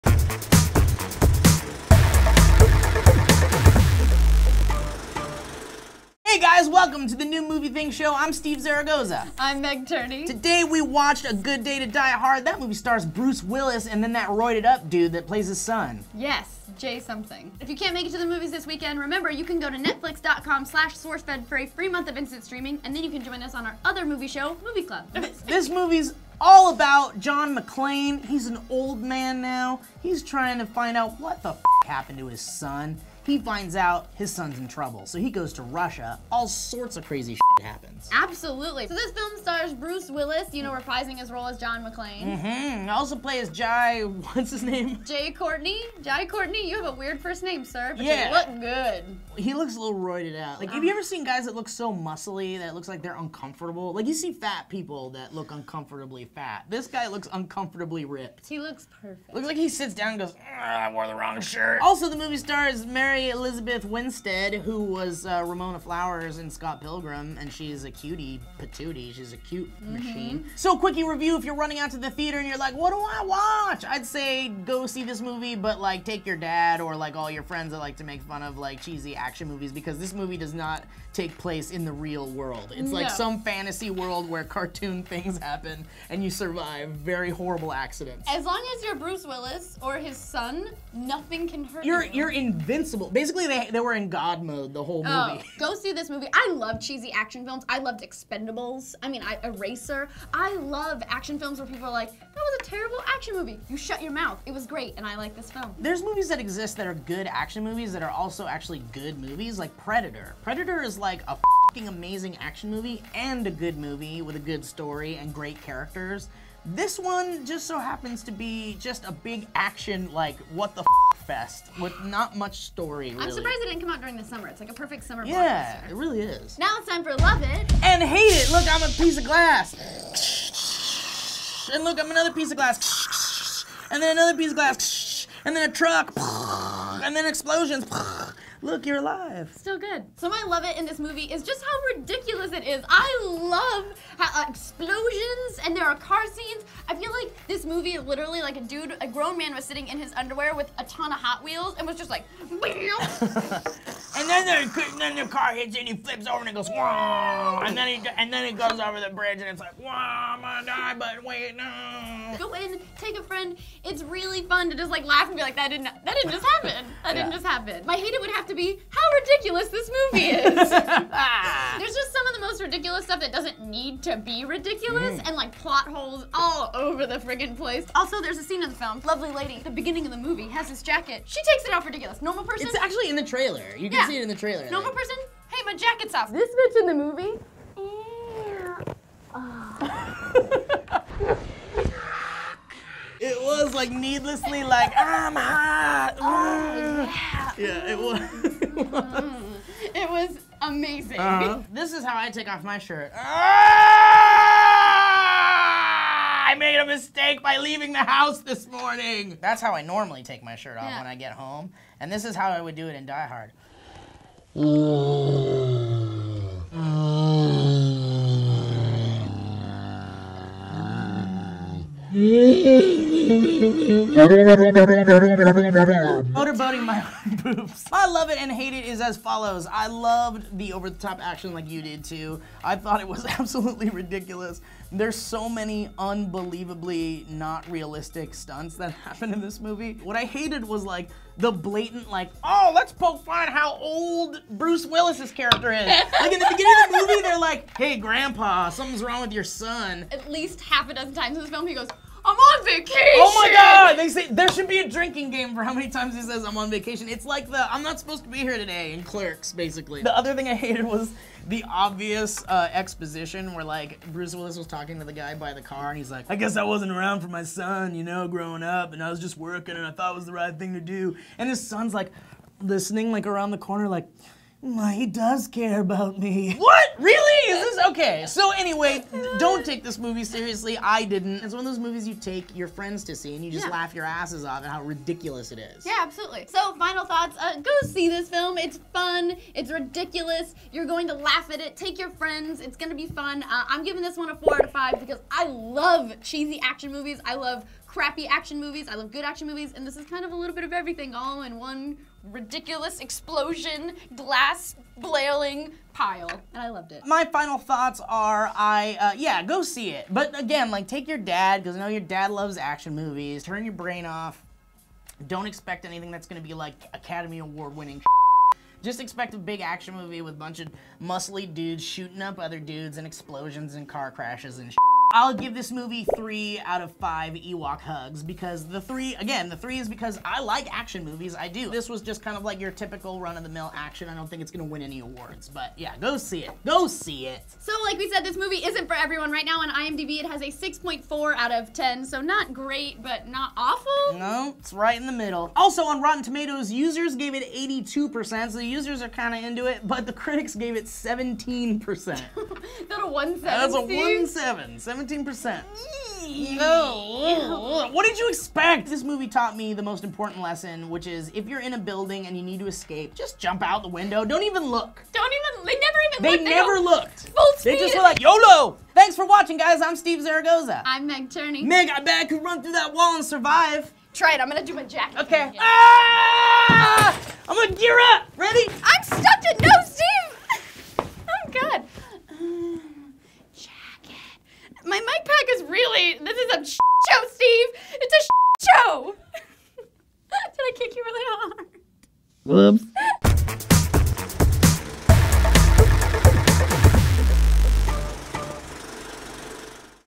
A Good Day to Die Hard - Reviewed
Bruce Willis sets down his walker to kick some Russian ass (and bond with his son) in this week's New Movie Thing Show!